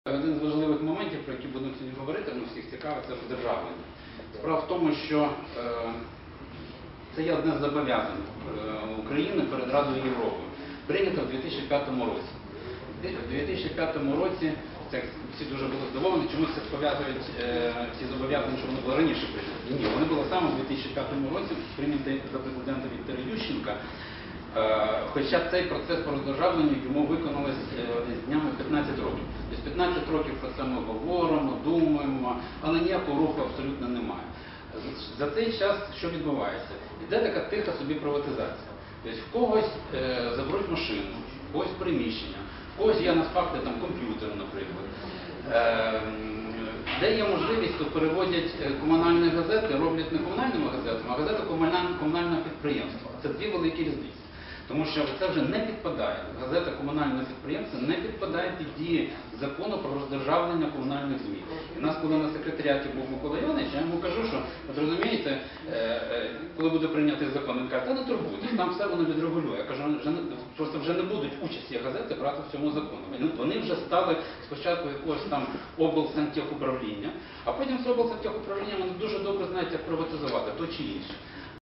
Олег Наливайко: "Роздержавлення медіа -- обов'язок України перед Радою Європи".
21 березня до Чернівців завітав голова Національної спілки журналістів. На прес-конференції він розповів про основні проблеми засобів масової інформації та зазначив, що роздержавлення є однією з найактуальніших.
Вікторія Яшан